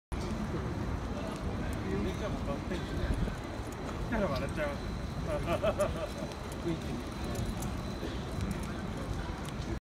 2018.4.11 船橋🐎10R リンゴカン
エイプリルダッシュ サラ系B3 C1選抜馬 （ハンデ）1000m
リンゴカン（牡6歳）468kg（+5kg）14頭立て5番人気4着